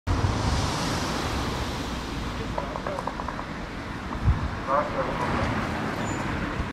პარკინგი 'ზებრაზე' – პატრული დარღვევას ‘ვერ ამჩნევს’
ახალციხეში, ნათენაძის ქუჩაზე,  ქვეითად მოსიარულეთა გადასასვლელზე გაჩერებულ მანქანას საპატრულო პოლიციამ ჩაუარა, თუმცა მათი მხრიდან დარღვევას რეაგირება არ მოჰყლია.